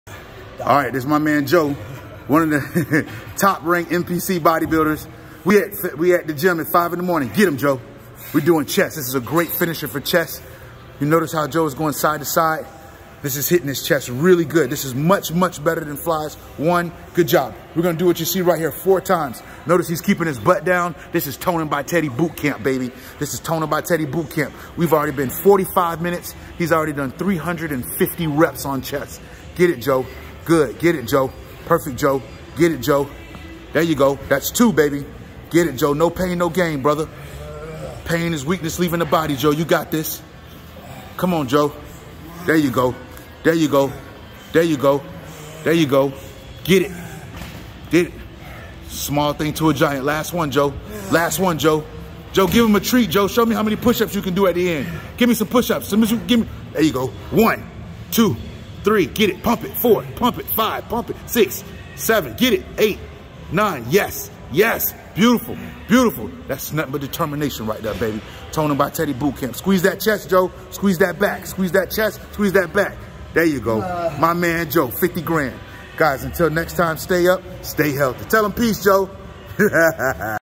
The ToningByTeddy Chest Crawl is something serious
Joe (a top ranking Bodybuilding athlete) is performing a killer chest crawl exercise after completing 350 reps on everything from machine chest and incline press, dumbbell flies abs comb, and 3 tricep exercises. He accepted the toningbyteddy chest challenge and won but he almost threw up twice 😳😅 This is great for putting volume into your workout to help ensure you get big chest gains